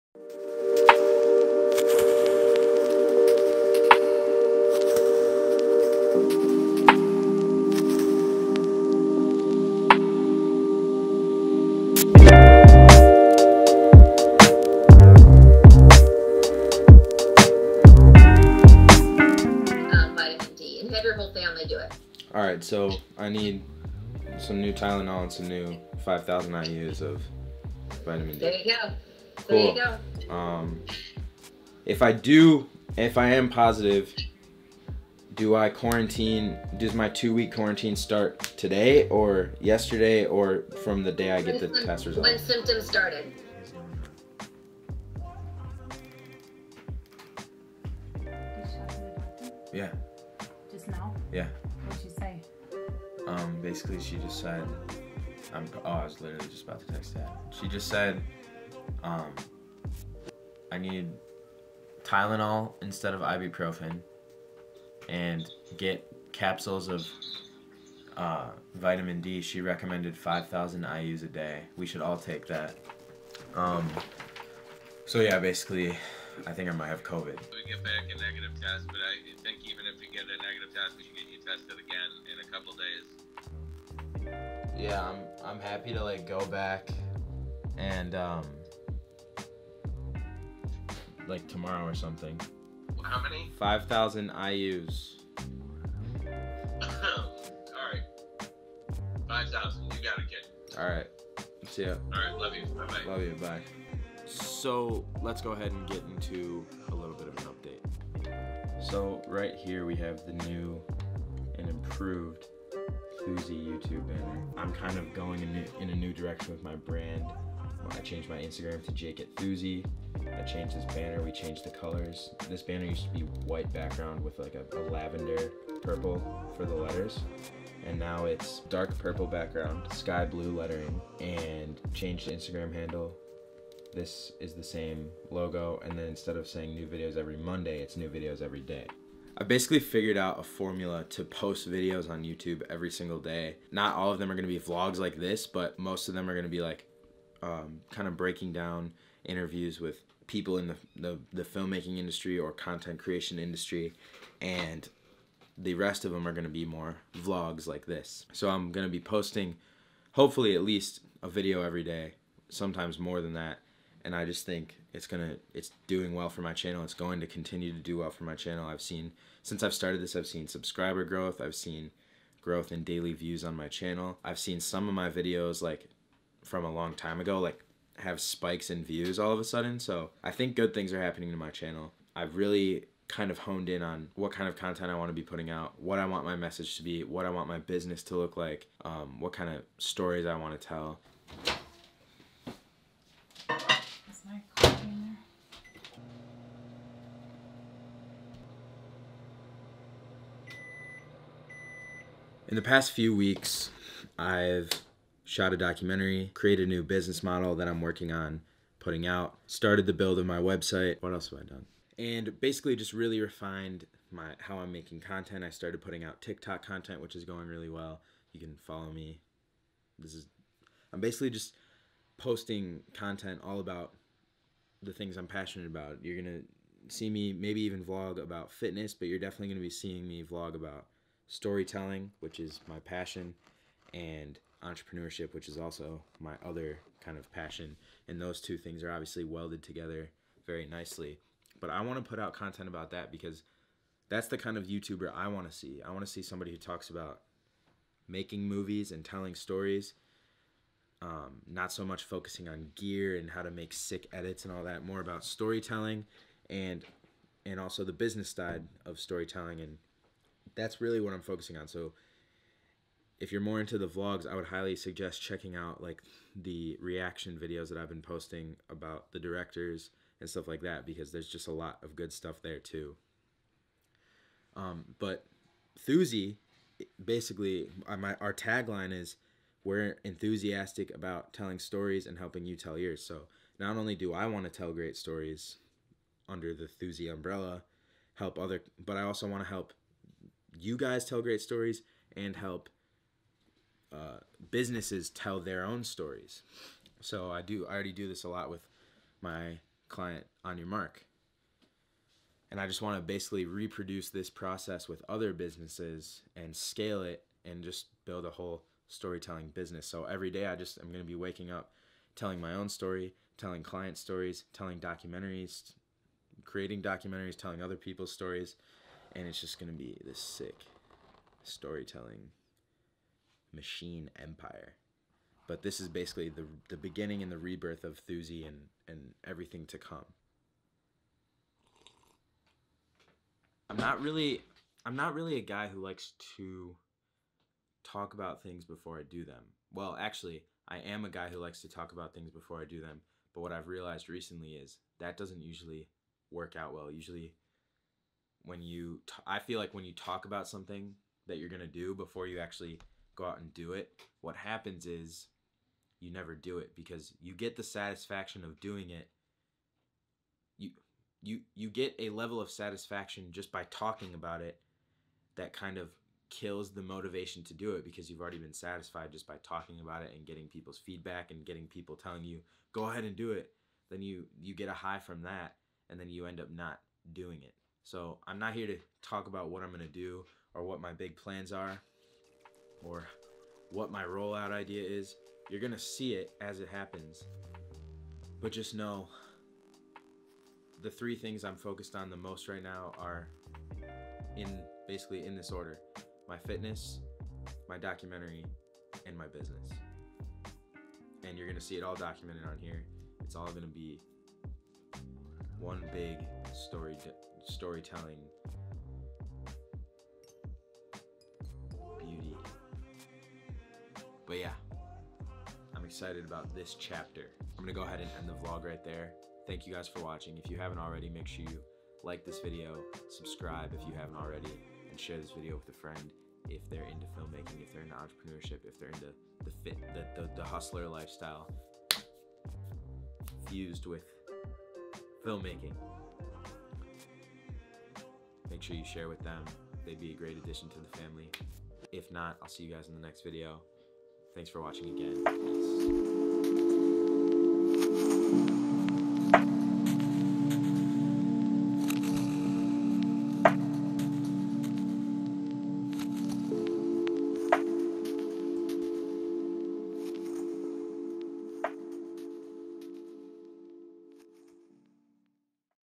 I Might Have COVID-19 | Life Update
Locked in the house because I might have covid-19. I decided I'd make a vlog and do a little life update because I have a lot of projects in the works.

Thanks again!